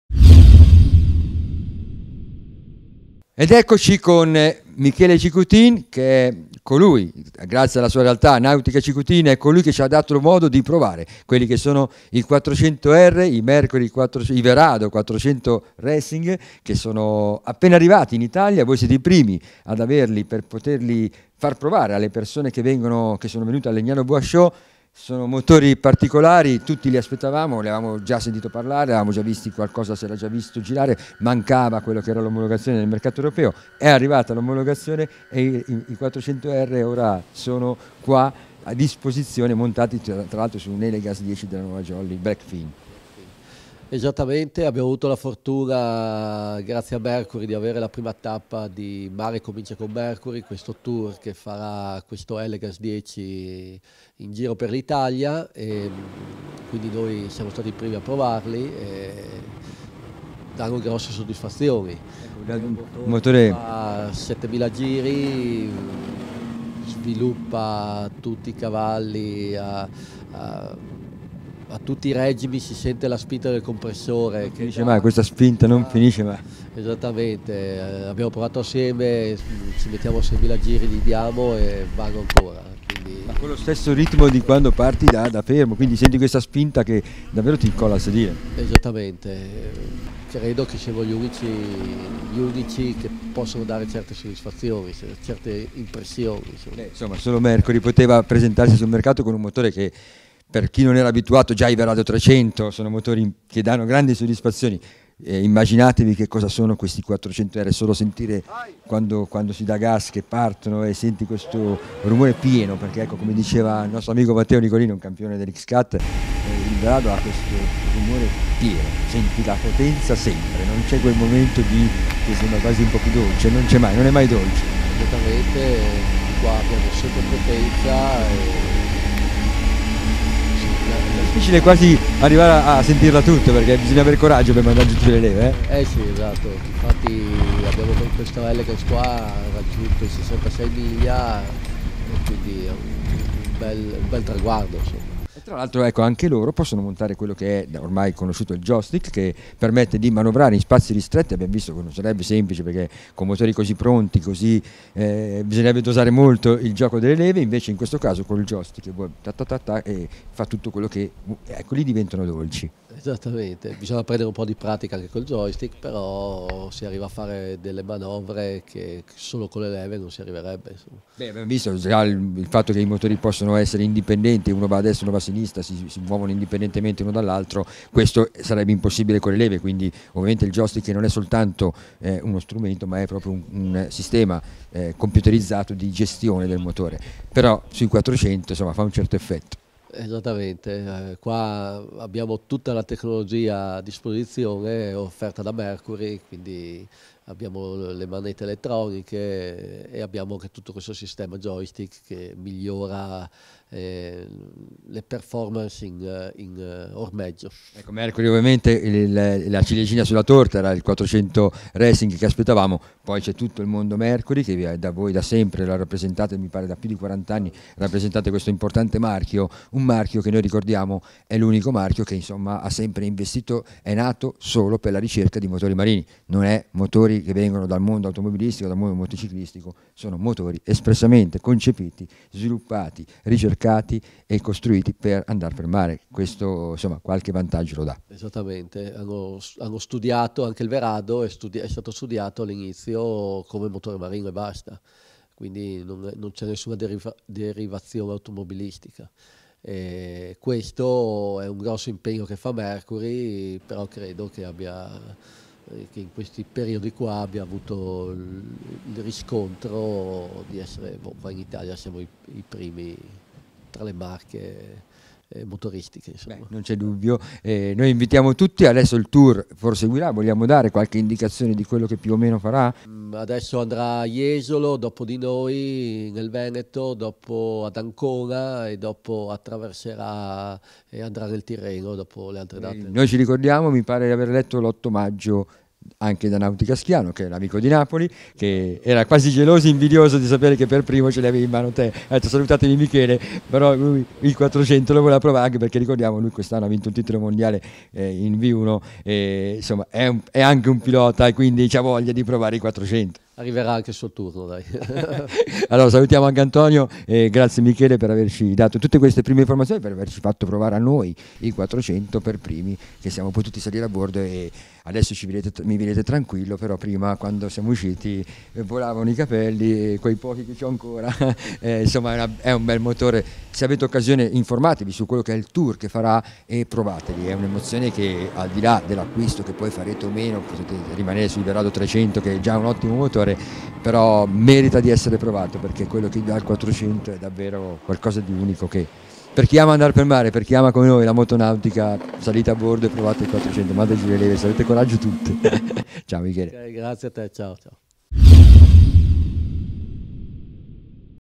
Mercury Verado 400 Racing by Nautica Cicuttin
prima uscita ufficiale dei nuovissimi e potentissimi Verado 400 R che ora faranno il giro dell'Italia presso diversi concessionari Mercury